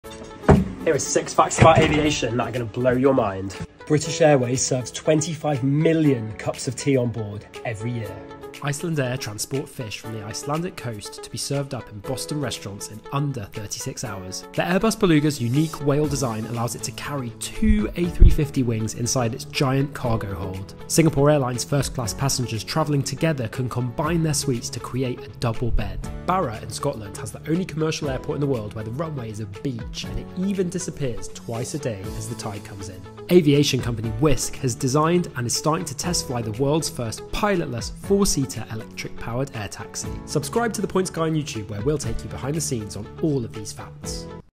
This Airbus holds WHAT?! Six Unique Aviation Facts
Did you know these aviation facts? ✈️ There’s a lot more where that came from…